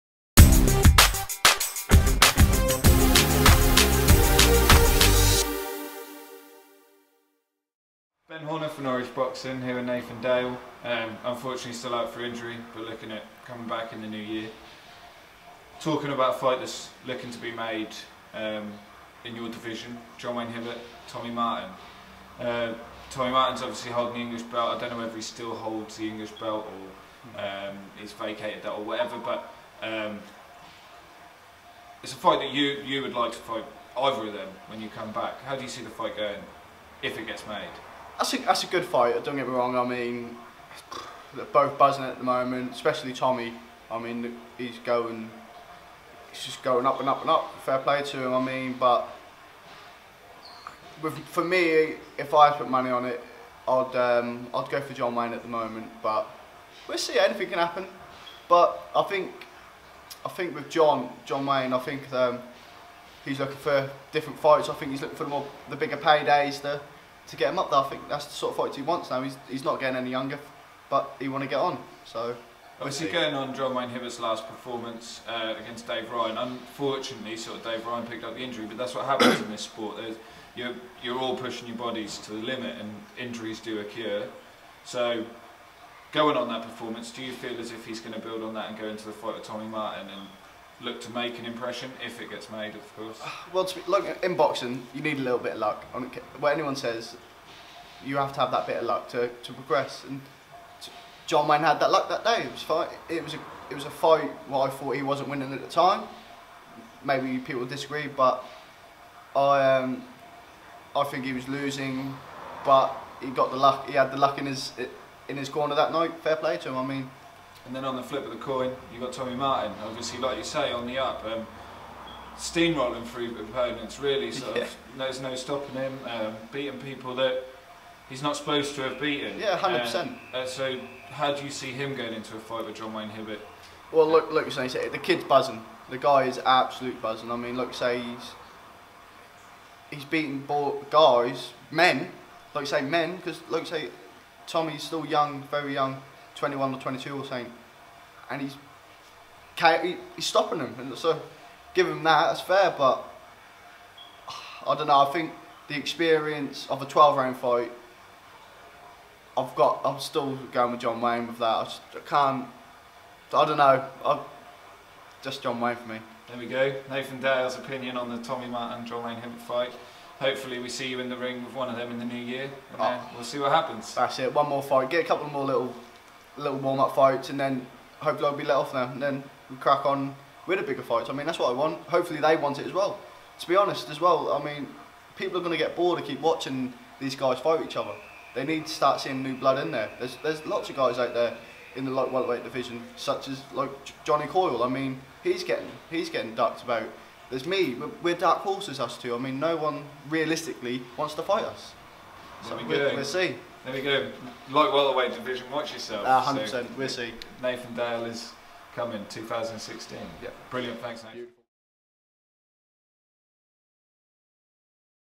Nathan Dale gives his views on Tommy Martin and John Wayne Hibbert
In association with Shamrock Promotions.